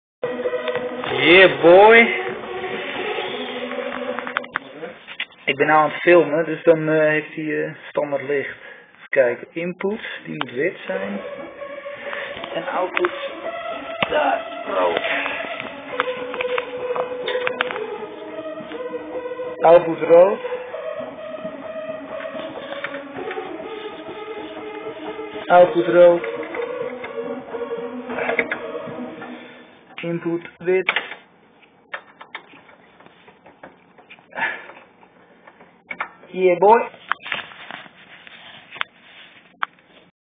Ready to connect!
Alex B connecting some wires in the studio so we can continue our project in a proper way.